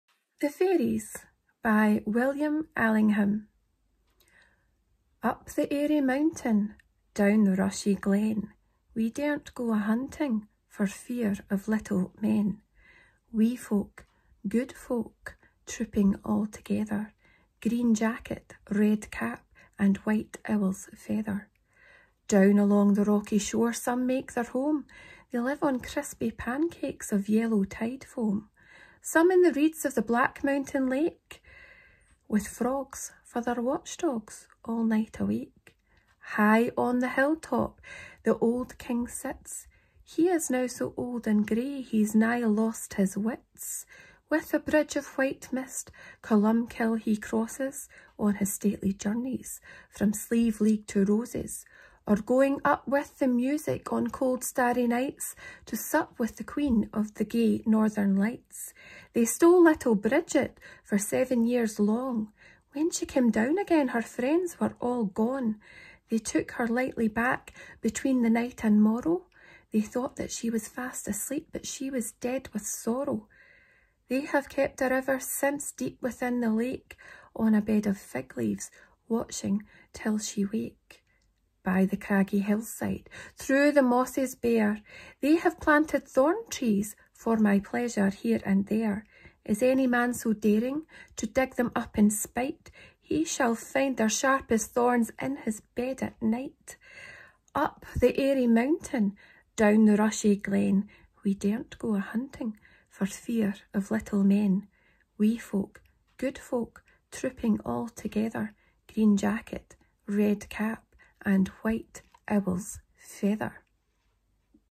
‘The Fairies’, William Allingham
‘The Fairies’ by William Allingham
Read by Natasha Sinclair

Up the airy mountain,
Down the rushy glen,
We daren’t go a-hunting
For fear of little men;
Wee folk, good folk,
Trooping all together;
Green jacket, red cap,
And white owl’s feather!

Down along the rocky shore
Some make their home,
They live on crispy pancakes
Of yellow tide-foam;
Some in the reeds
Of the black mountain-lake,
With frogs for their watchdogs,
All night awake.

High on the hill-top
The old King sits;
He is now so old and grey
He’s nigh lost his wits.
With a bridge of white mist
Columbkill he crosses,
On his stately journeys
From Slieveleague to Rosses;
Or going up with the music
On cold starry nights,
To sup with the Queen
Of the gay Northern Lights.

They stole little Bridget
For seven years long;
When she came down again
Her friends were all gone.
They took her lightly back,
Between the night and morrow,
They thought that she was fast asleep,
But she was dead with sorrow.
They have kept her ever since
Deep within the lake,
On a bed of fig-leaves,
Watching till she wake.

By the craggy hillside,
Through the mosses bare,
They have planted thorn trees
For my pleasure, here and there.
Is any man so daring
As dig them up in spite,
He shall find their sharpest thorns
In his bed at night.